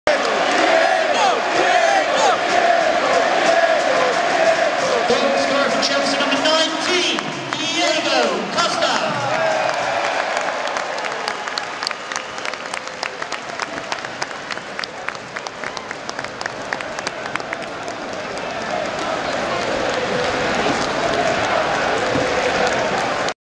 Chelsea - Southampton - Chelsea celebration after scoring
Diego Costa celebration after gol